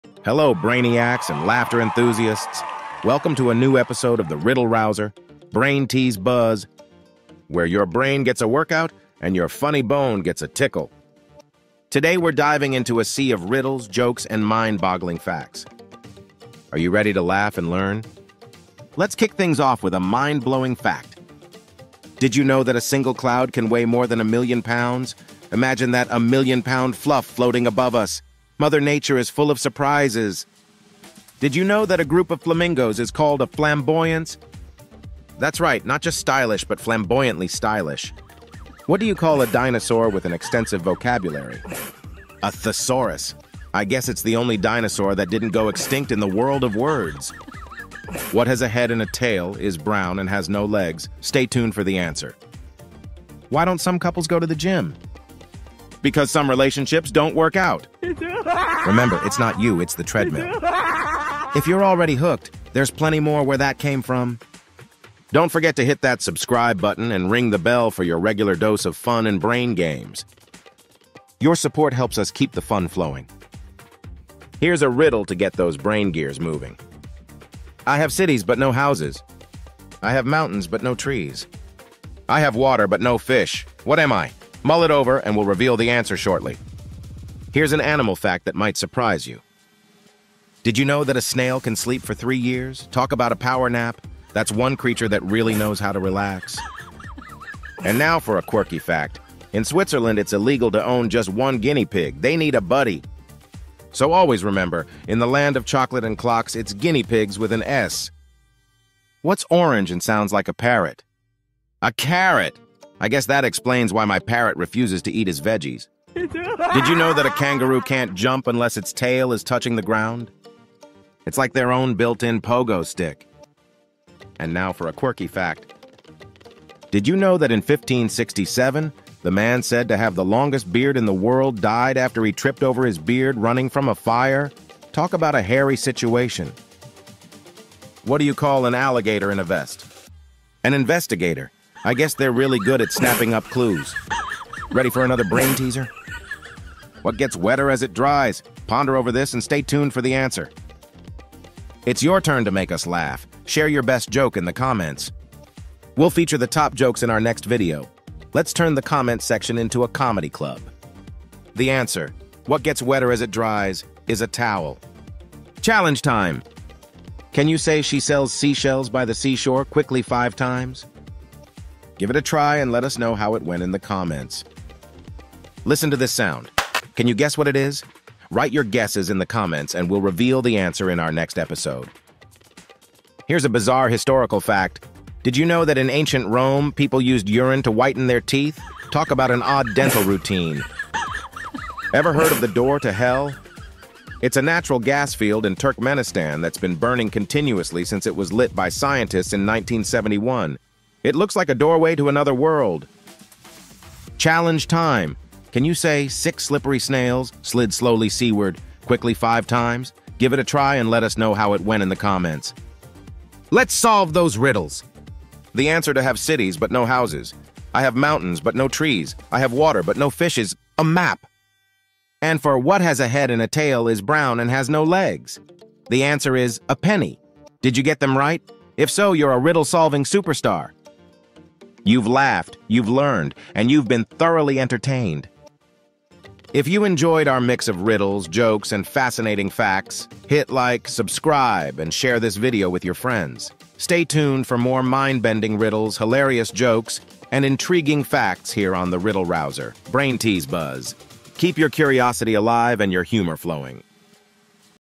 The Riddle Rouser's Extravaganza of Puzzles and Punchlines!
🧠💡 "Get ready to embark on a rollercoaster ride of laughter and brain teasers with The Riddle Rouser - BrainTeaseBuzz! In today's episode, we dive into a whirlwind of riddles, jokes, and mind-boggling facts that will challenge your intellect and tickle your funny bone. From the mysteries of nature to historical oddities, we've got a blend of entertainment and education that you won't want to miss.

🤔 Can you solve our brain-twisting riddles before we reveal the answers? Do you have what it takes to be the Riddle Champion? Join us for a journey filled with fascinating animal facts, historical curiosities, and a mystery sound game that will keep you guessing.

Your support helps us bring more exciting content to your screen.